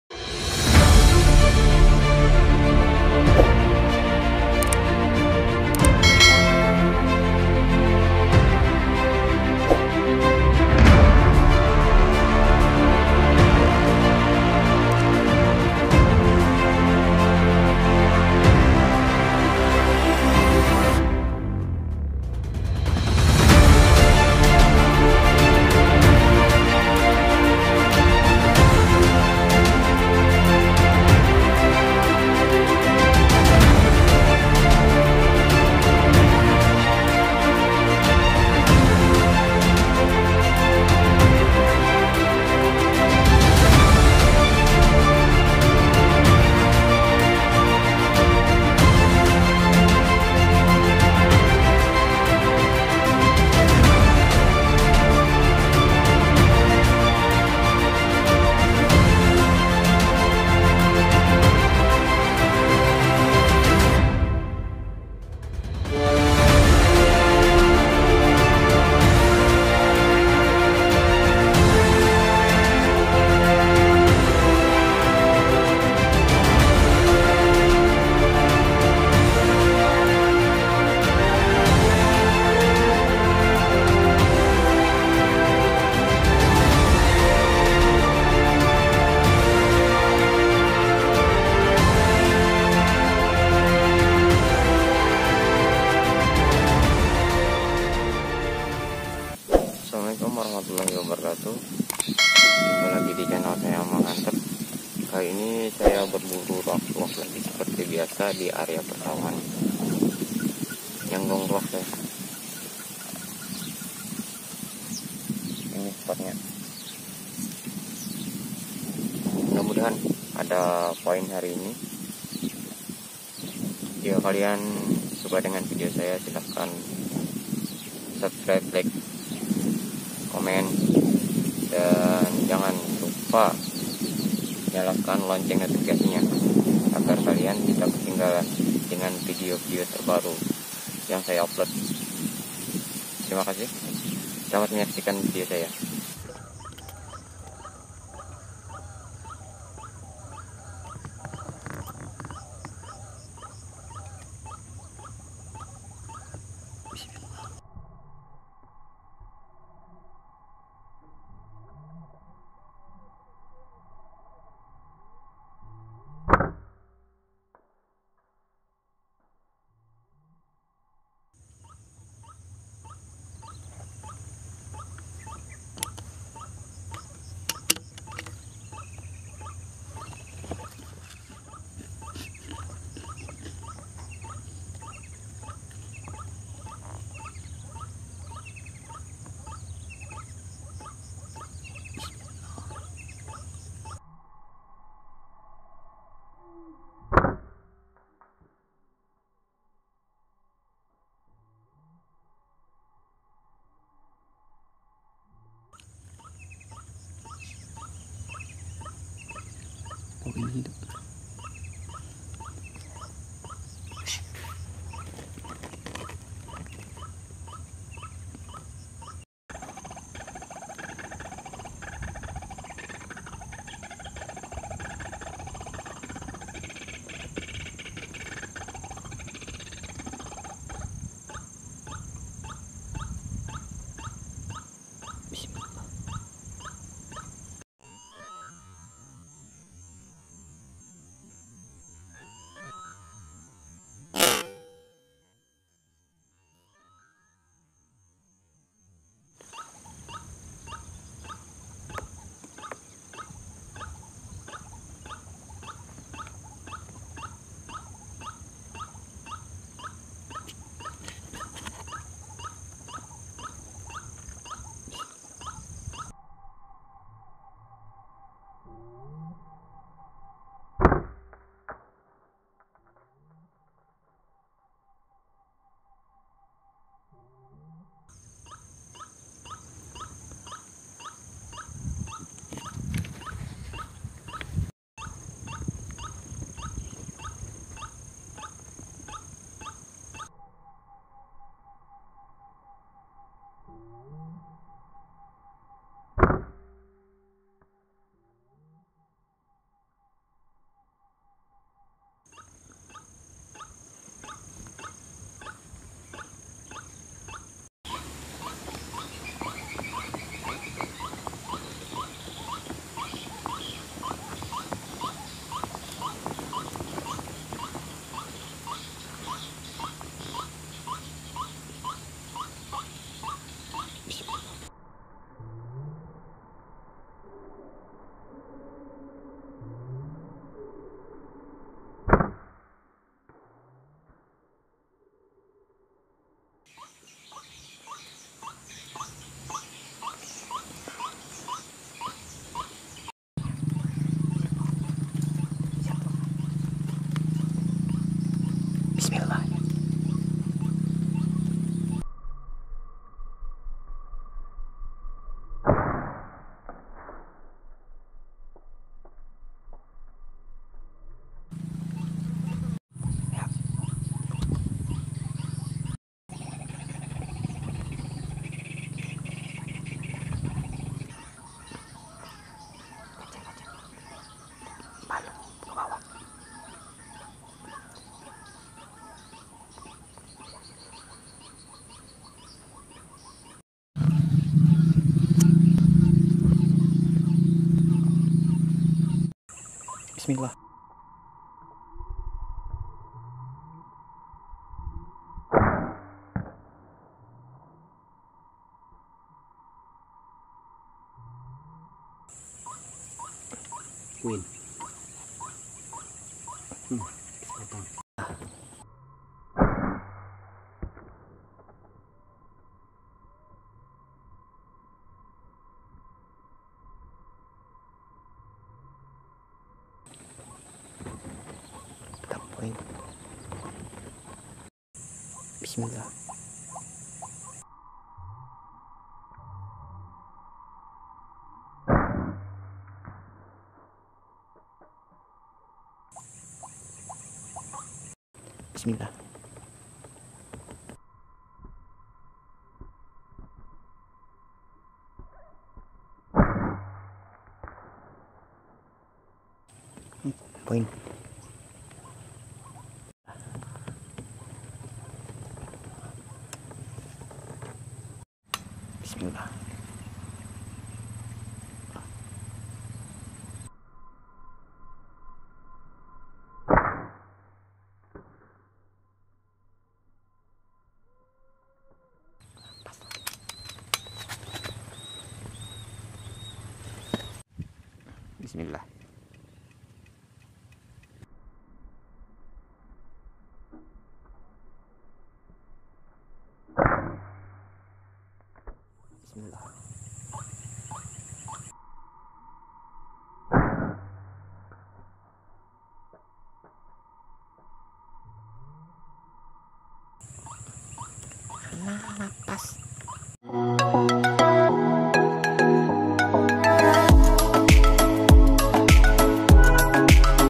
🔴BERBURU BURUNG RUAK RUAK KETEMU BIAWAK
kawan-kawan diseluruh nusantara, semoga diberi kelancaran dalam setiap usaha kita serta dilimpahkan kesehatan dan rejki🤲